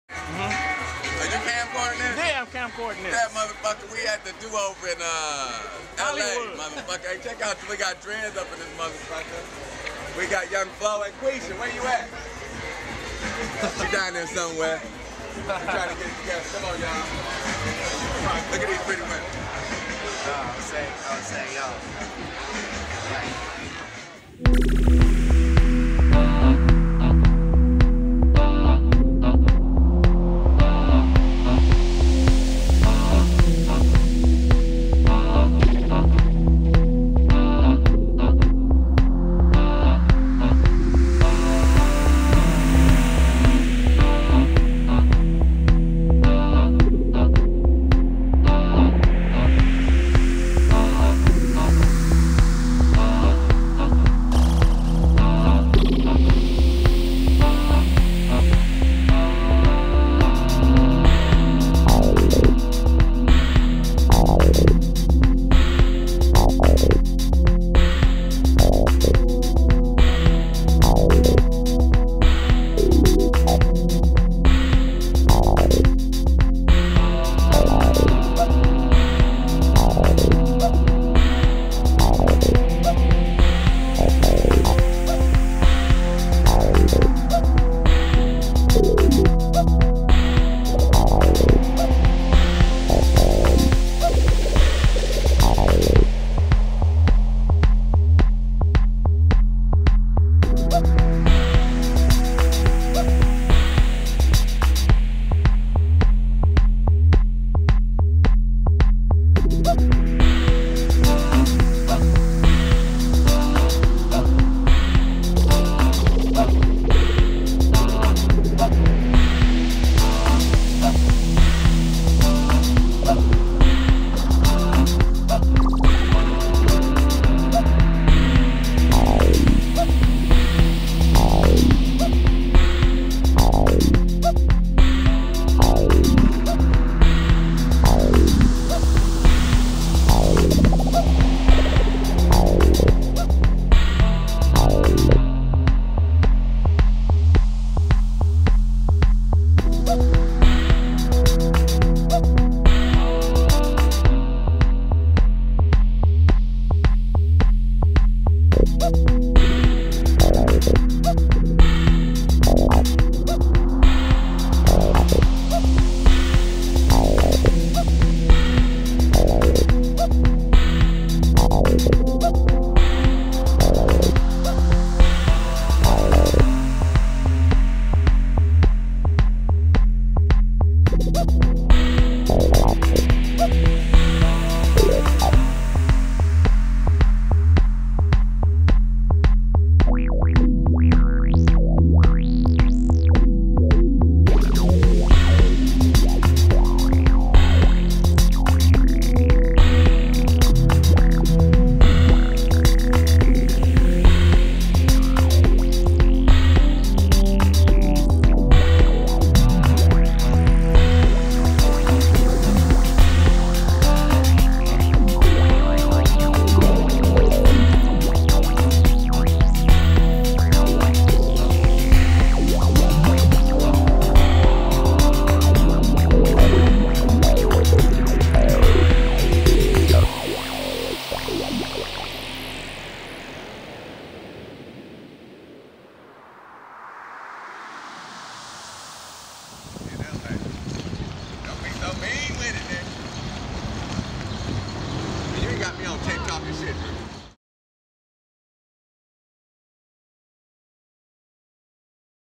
MOODYMANN presents RADIO
Detroit invades LA ... Moody popped up and surprised everybody at the exclusive  Do Over Party, which is well known for not promoting their guests to the public. This time he pulled one over on the promoters, popped out of nowhere to share the stage with Flying Lotus. Check out the visuals, filmed on the day of the party...